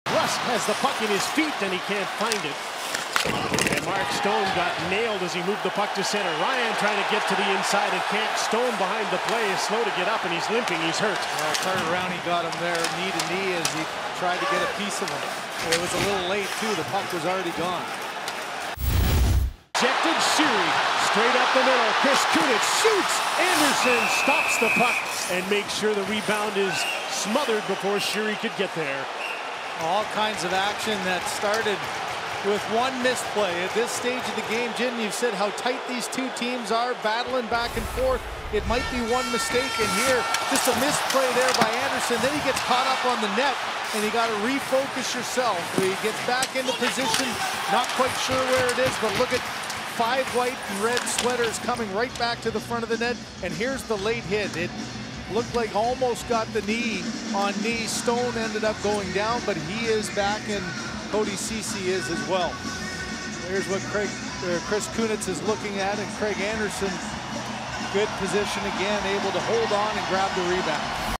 Stone exits quickly after collision with Rowney
Ottawa Senators forward Mark Stone went down the tunnel in a hurry after his collision with Pittsburgh Penguins forward Carter Rowney.